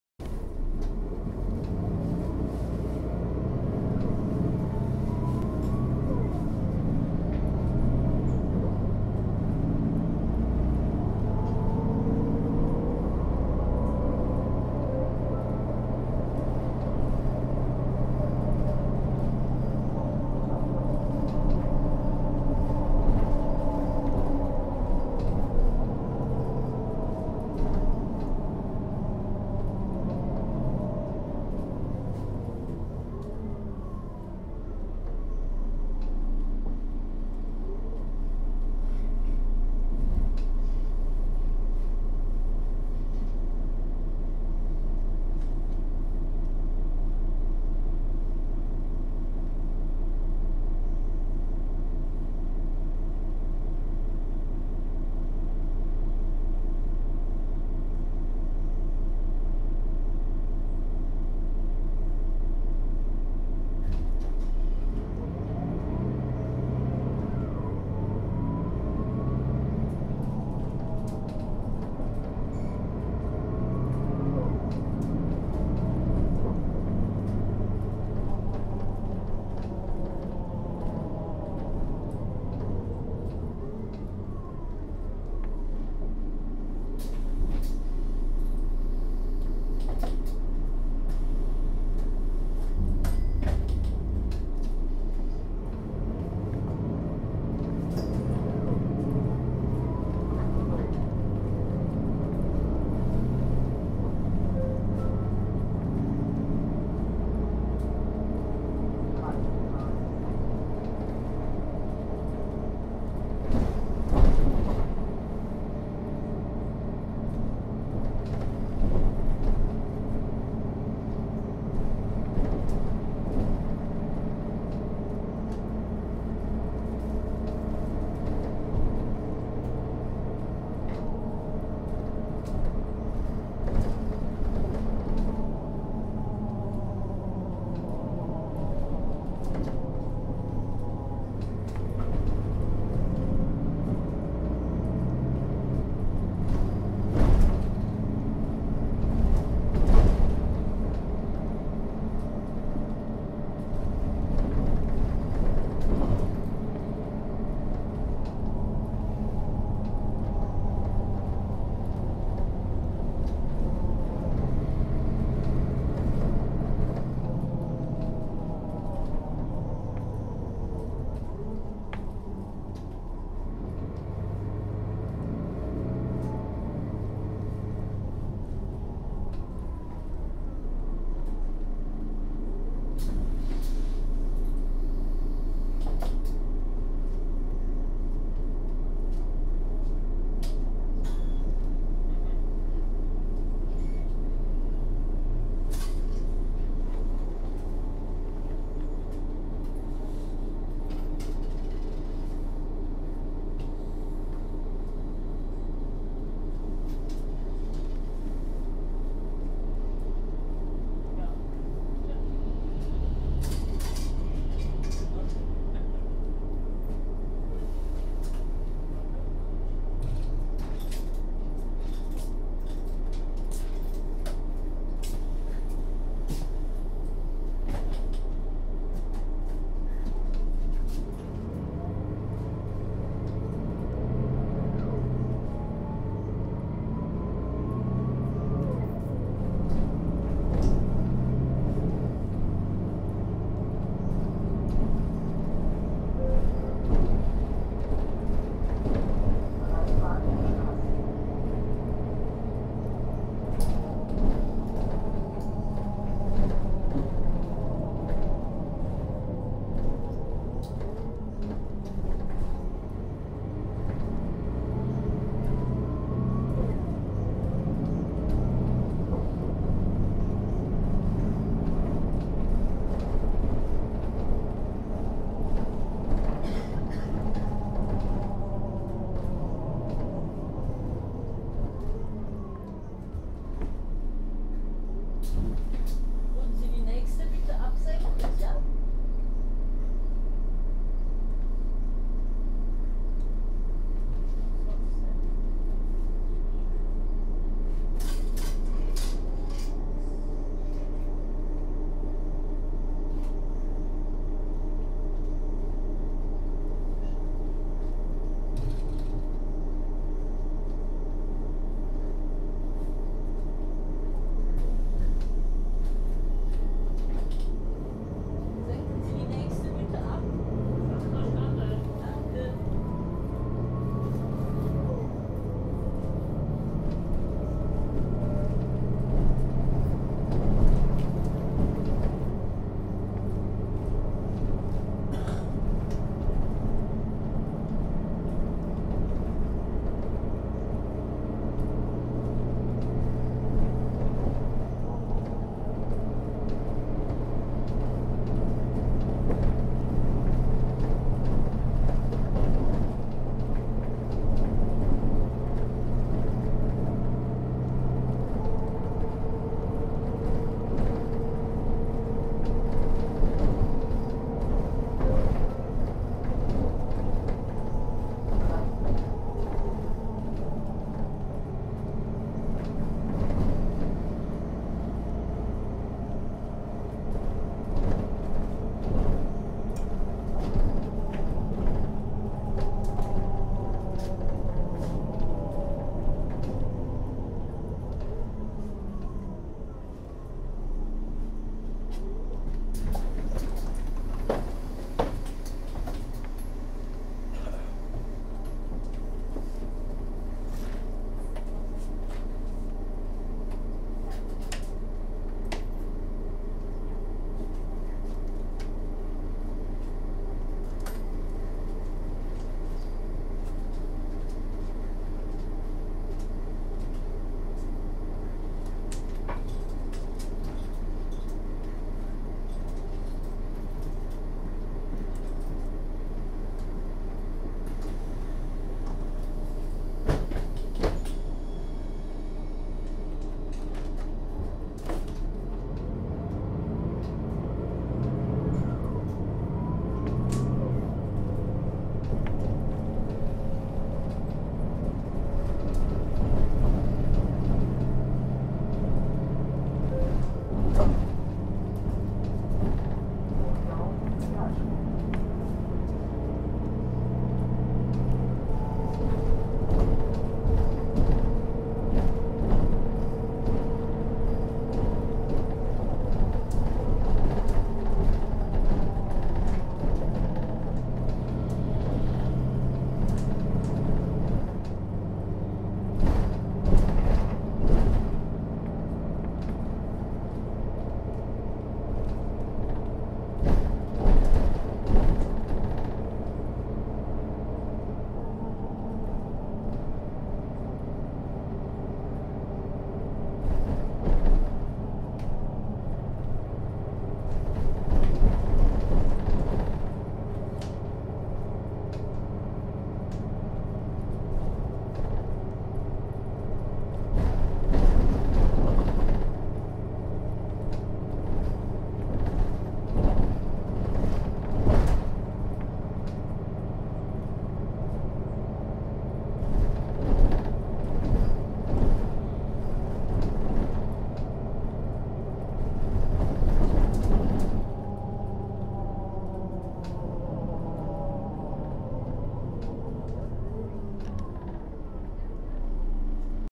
[Sound] Bus Mercedes-Benz O 530 der Fa Bernd Kolf GmbH
Bus: Mercedes-Benz O 530
Kennzeichen: SU-K 610
Firma: Bernd Kolf GmbH, Eitorf
Baujahr: -
Wagennummer: -
Anzahl: 9
Aufgenommen auf der Linie: 550
Von: Rheidt Nord
Nach: Mondorf Realschule